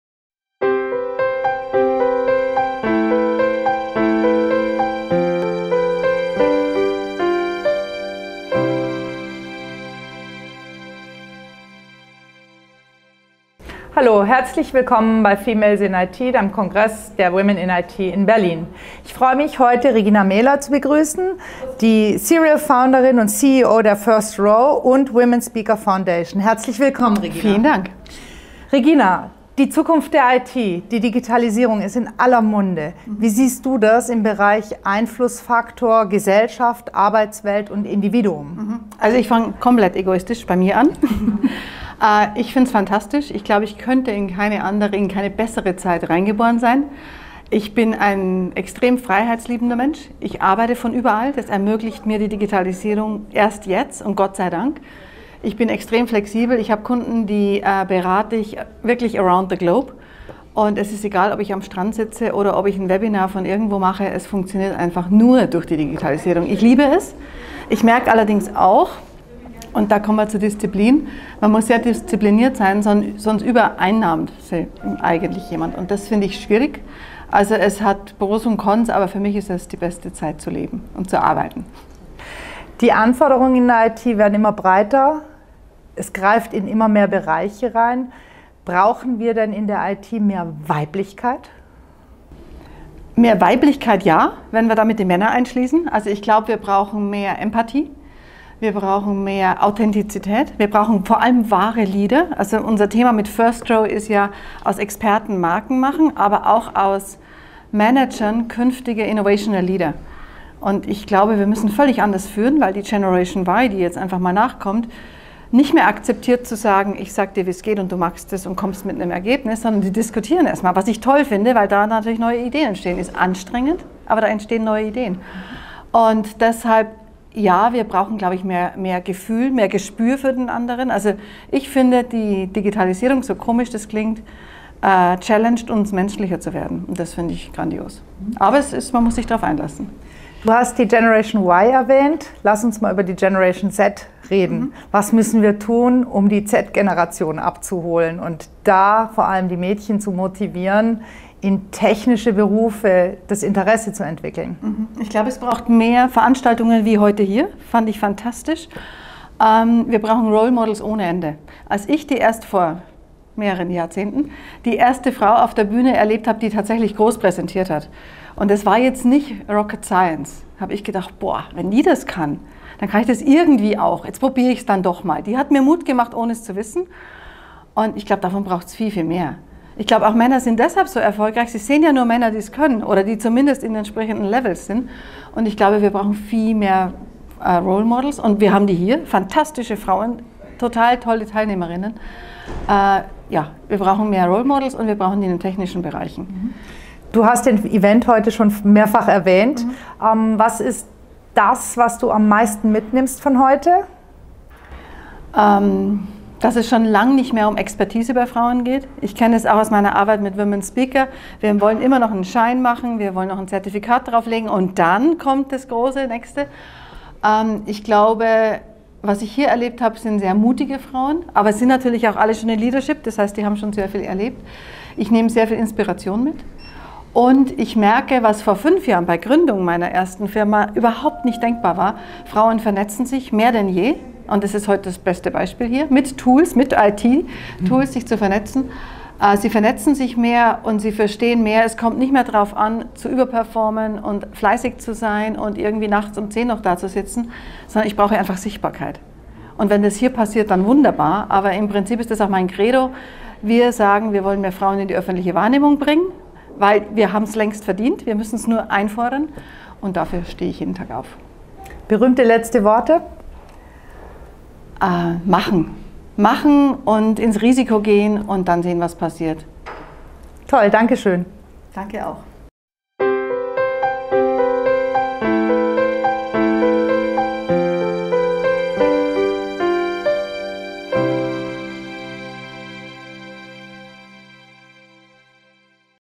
Interview mit Regina Mehler, Unternehmerin, Autorin und Gründering der WOMEN SPEAKER FOUNDATION
Das Interview wurde auf dem FIT-Kongress 2017 aufgezeichnet.
*Urheber ist Harmann Fotografie